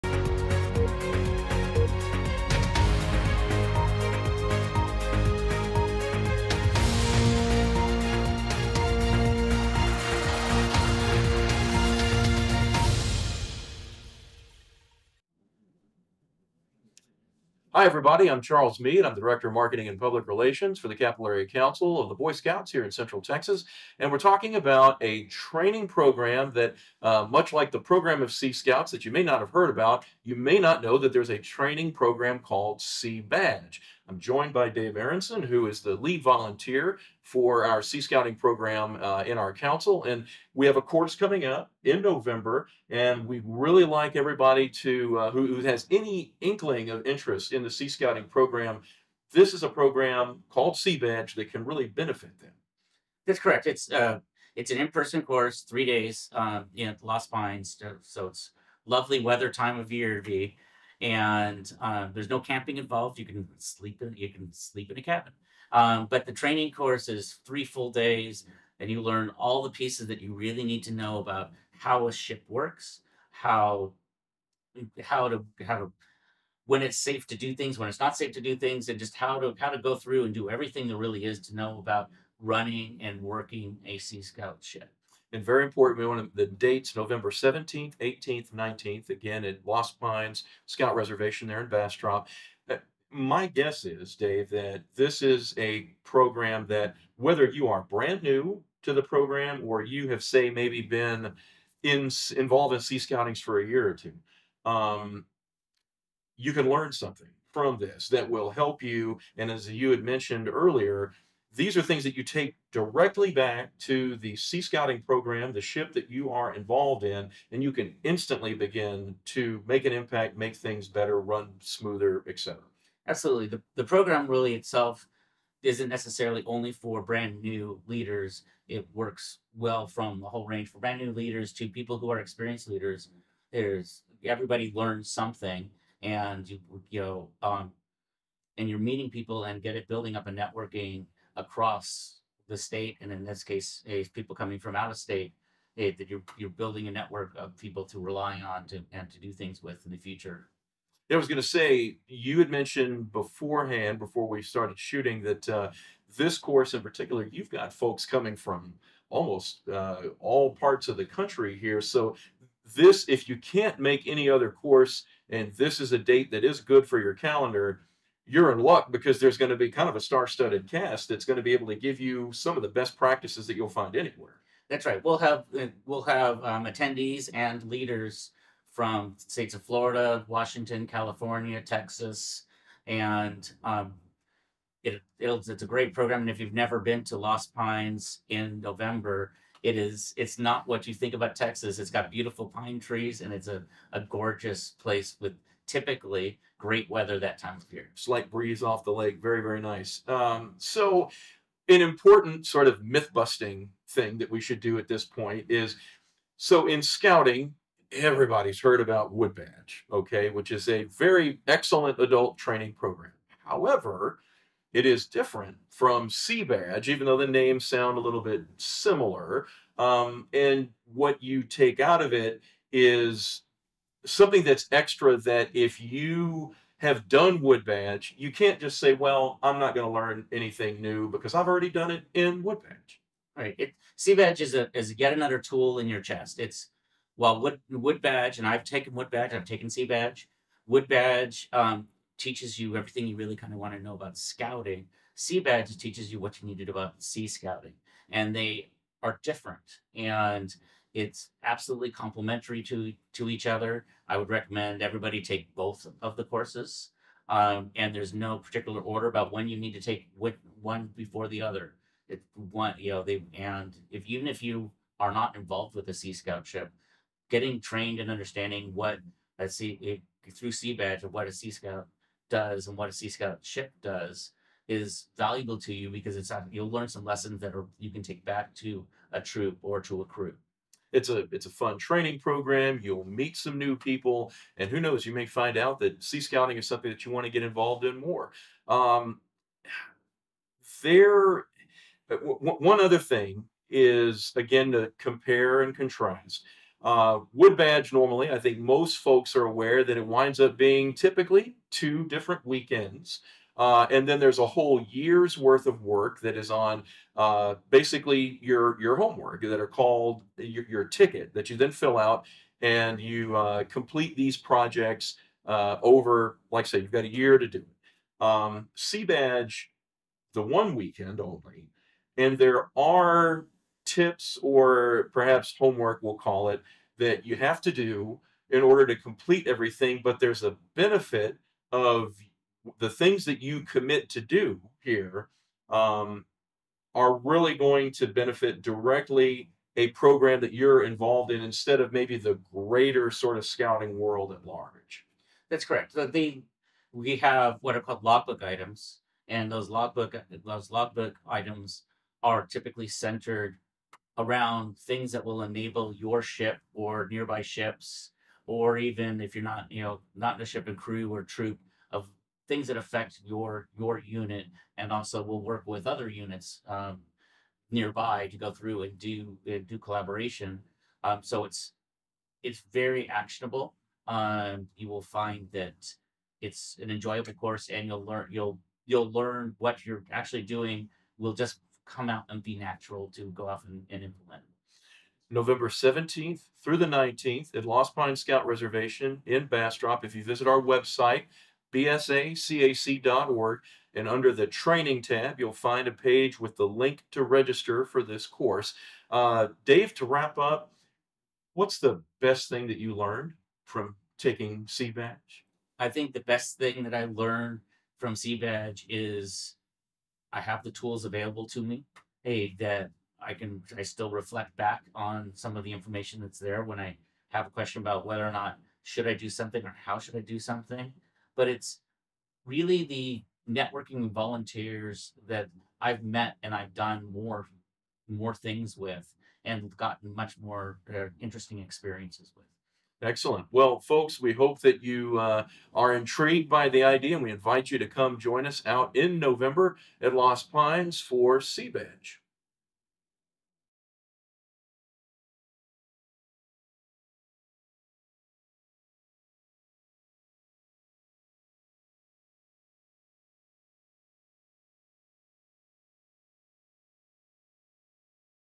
What is Seabadge?
Charles Mead, Council Director of Marketing and Communications, and Dave Aronson, Council Sea Scout Committee Chair discuss the Seabadge leadership course. [Seabadge-72-TX-2023 ] being held in person at LSPR, Nov. 17, 18, and 19.